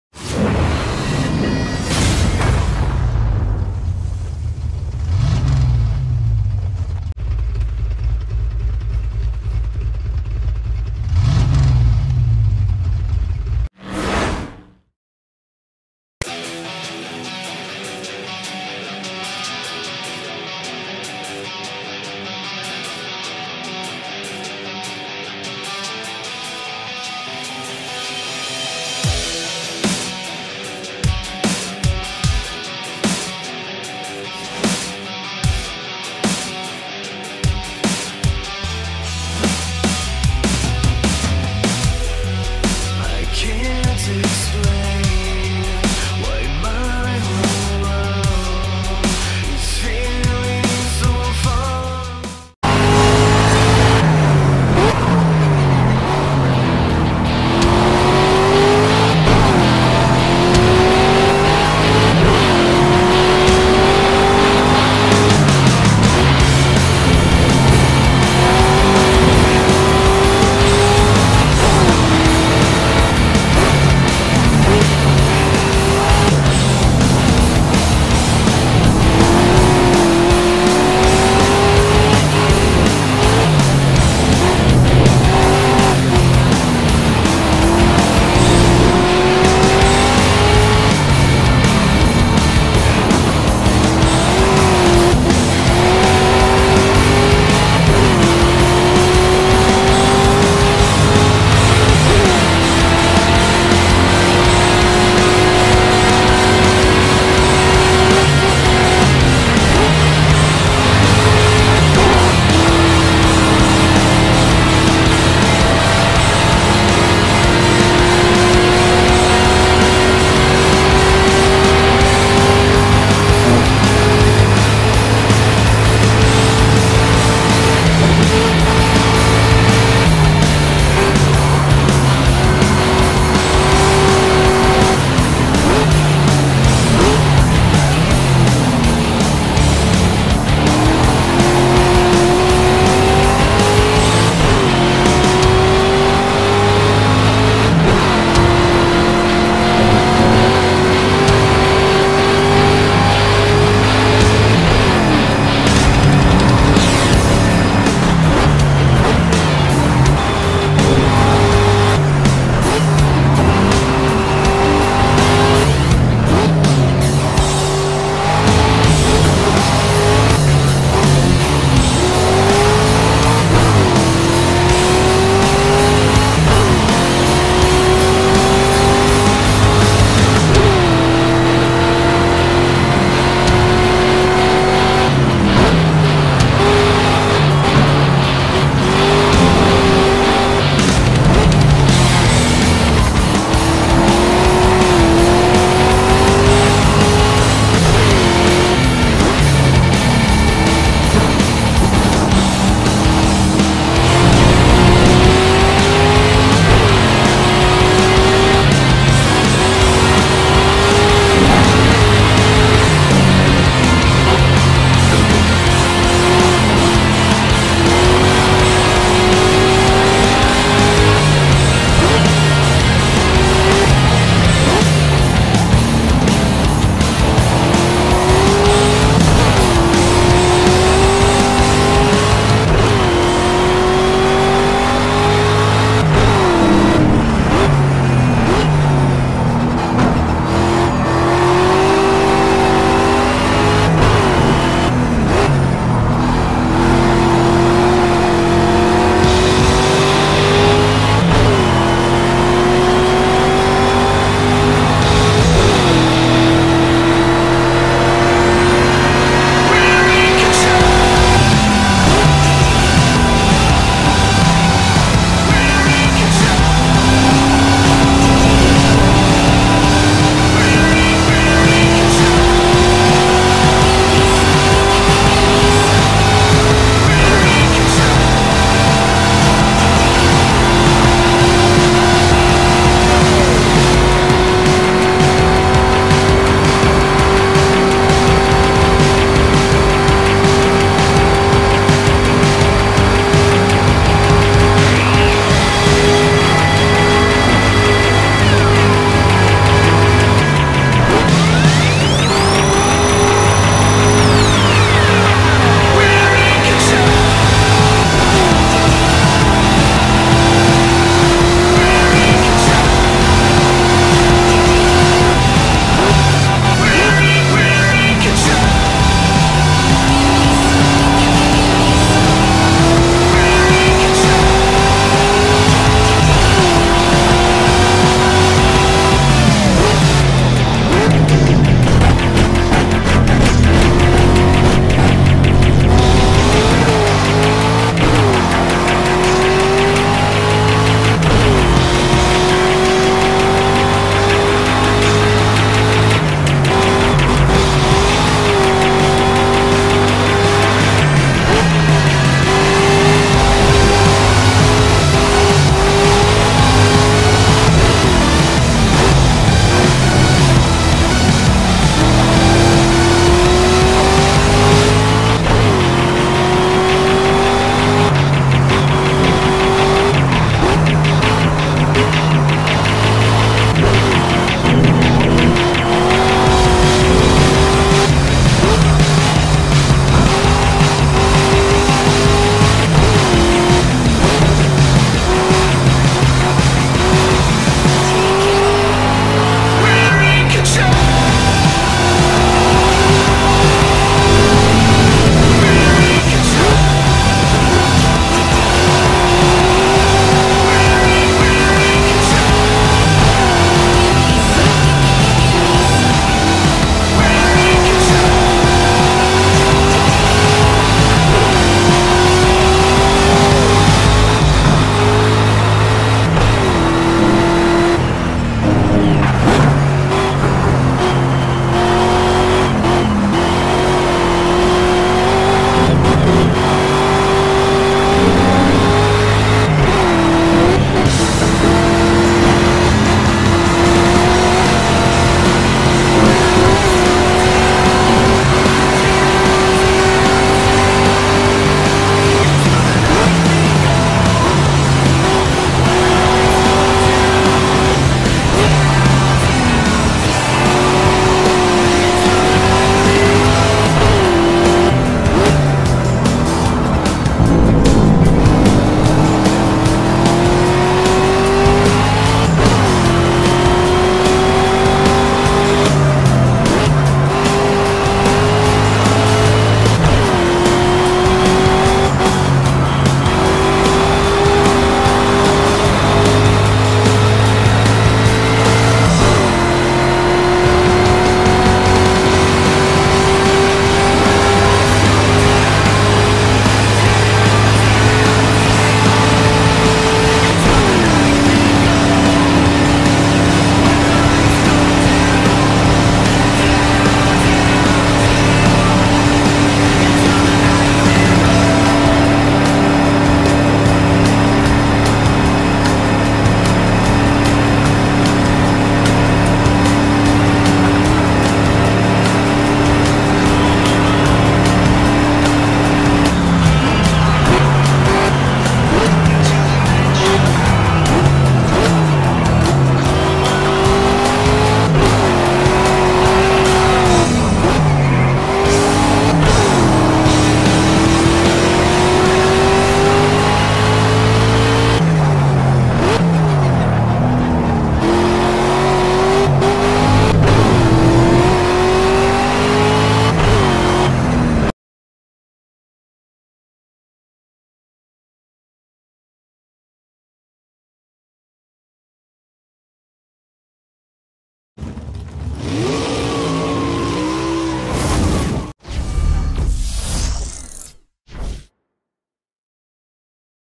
2015 Mustang GT @ Nurburgring
2015 Mustang GT @ Nurburgring                                                                                                                                                                                                                                                  using a T500RS & TR8A no aids only abs and with no changes to the car absolutely stock